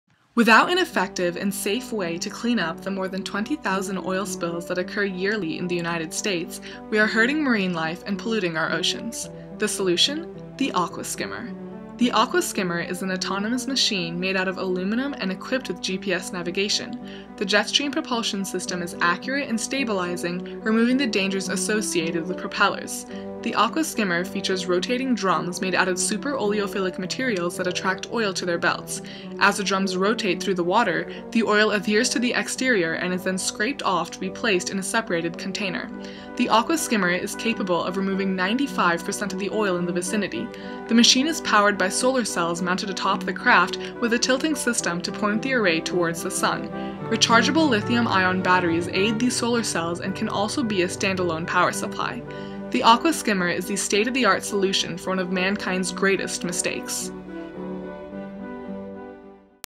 The AquaSkimmer (VEX Robotics Online Autodesk Sustainable Design Challenge)
This is the video component of our VEX Robotics Online Autodesk Sustainable Design Challenge.  We call it the AquaSkimmer.

Created by 10D Exothermic Synergy (members: Eric Burlingame, Samhita Karnati, Nikhil Auradkar, Lila Balakrishnan, Leanne Su, and Alex Zhou)